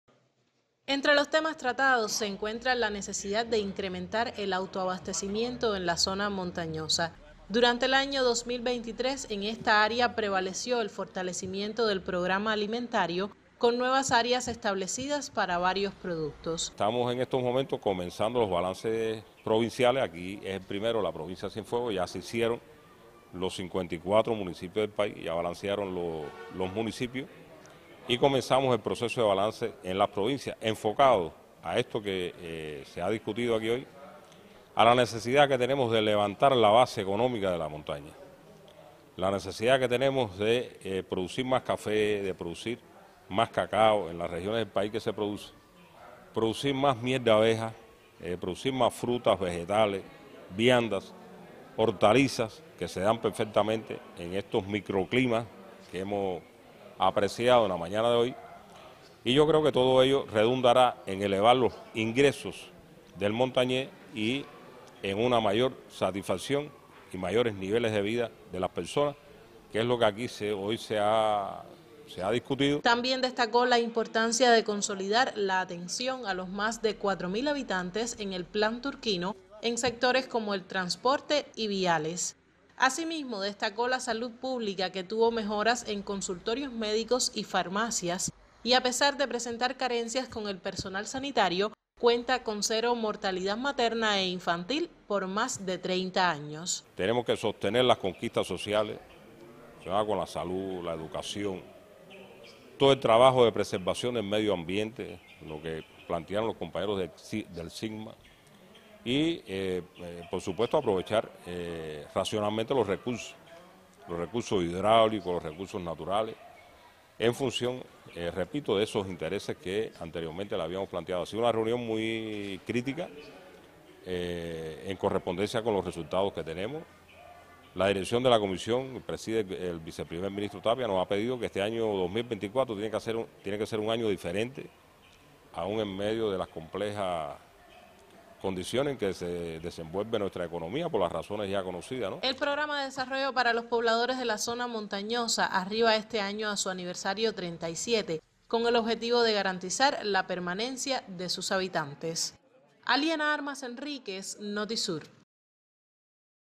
Analizan resultados del trabajo en Plan Turquino de Cienfuegos
En un recorrido por asentamientos montañosos del lomerío de Cienfuegos se analizó el trabajo realizado durante el año 2023. El encuentro estuvo presidido por las máximas autoridades de la provincia y Santiago Pérez Caballero, Secretario de la Comisión Nacional de atención al Plan Turquino.